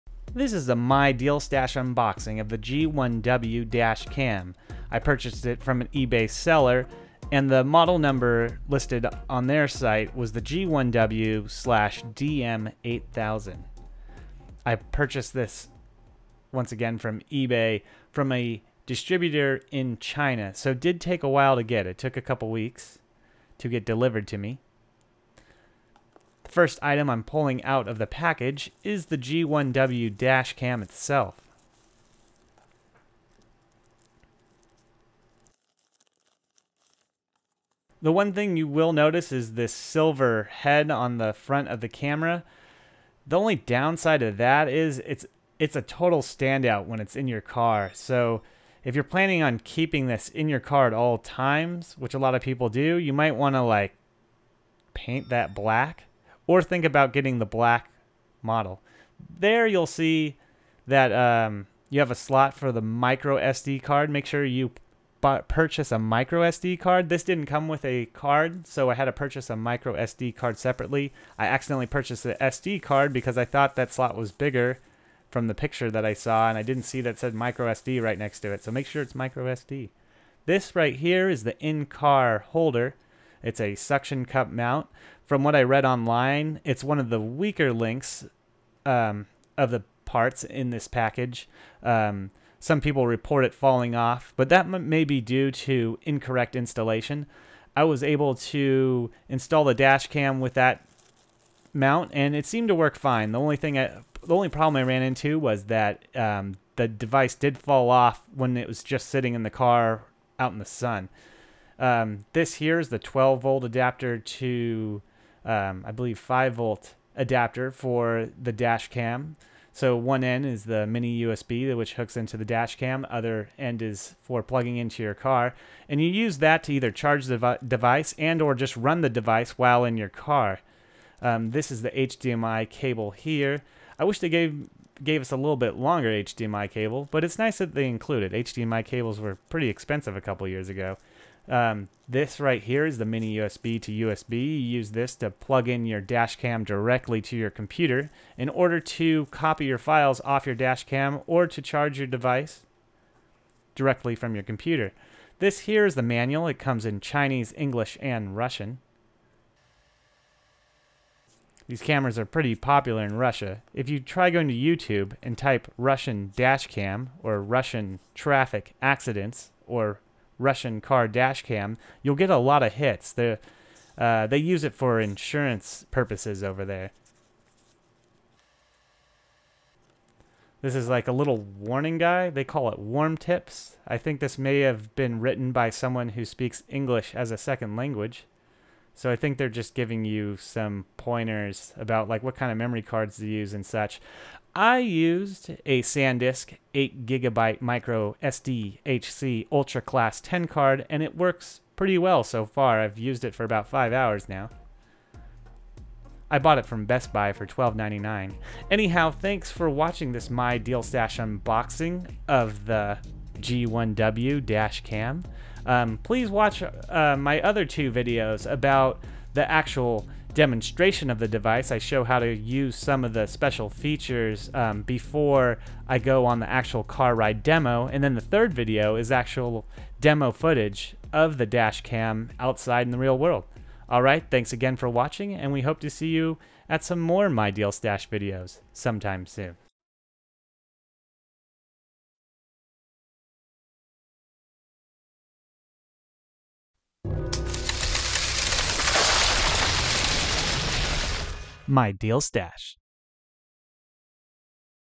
G1W Dash Cam Unboxing
Full HD 1080P G1W 2.7" LCD Car DVR Camera Recorder G-sensor H.264 Night Vision

Video Timeline
0:11 Opening the shipping package
0:23 Specs listed on the side of the G1W Box
0:37 G1W Dash Cam
1:02 Micro SD Card Slot
1:24 In-car holder  (suction cup mount)
1:45 In-car charging cable
2:08 HDMI cable
2:21 USB cable  (Mini USB to USB)
2:35 User Manual (Chinese and English)
3:00 Warm Tips??? : )
3:25 Credits

Made By Unkown (Blackview Generic) but is powered by the Novatek 96650 chip.
The model number specified by the ebay seller is: G1W/DM8000.

What is shown:
1 x G1W Dash Cam
1 x In-car holder  (suction cup mount)
1 x In-car charging cable
1 x HDMI cable
1 x USB cable  (Mini USB to USB)
1 x User Manual (Chinese, English and Russian)

Not Included but Needed:
1 x 4GB or larger Micro SD card.
I use a SanDisk 8GB Micro SDHC Ultra Class 10 card.
I bought the Micro SD card from Best Buy for $12.99.

Overview
Model:G1W
Chip:Novatek 96650
Resolution:
 1920x1080p*30fps;
Video Code format:MPEG4
G-sensor:Yes
Motion detection: Yes
GPS:No
LCD :2.7" Screen
Size:110x50x30mm
Day Video quality: Good
Night quality: Netural

What's in The Package
1 x G1W Dash Cam
1 x In-car charging cable
1 x In-car holder  (suction cup mount)
1 x USB cable  (Mini USB to USB)
1 x User Manual (Chinese and English)
1 x HDMI cable

Things you still need beyond what's included
1 x Micro SD Card (Choose a size 4GB, 8GB, 16GB or 32GB)
Memory card sizes larger than 32GB need special firmware updates to work but may void the warranty.

Related videos in this series
G1W Dash Cam Demo
G1W Dash Cam Driving Demo

Where to Buy the G1W
(Slightly different, black front to make it more covert for hard wire mounting)
(I bought from here.)

I bought mine from Ebay. Good price, 2 week shipping from China.